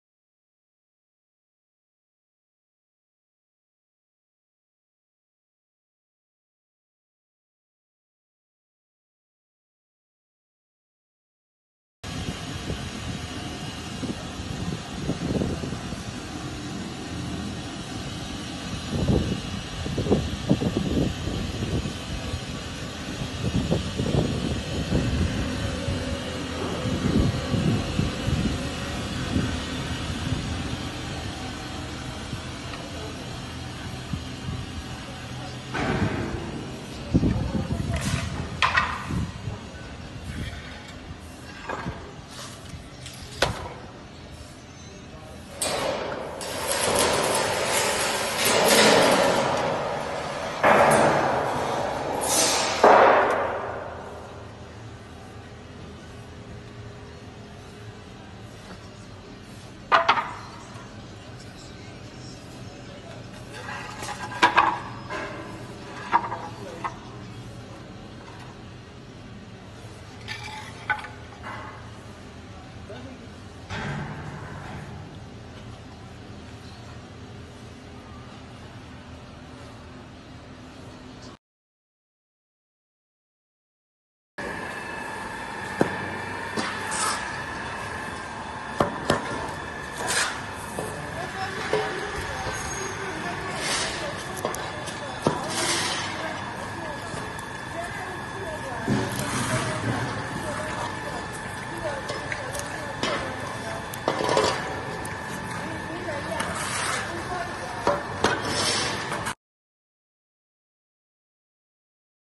How the Customized Vertical Hoist Carbonization Furnace Install and Work in client's plant?
Customized Vertical Hoist Charcoal Carbonization Furnace QHL-4 for client to make  wood charcoal.

Working in Client's working site.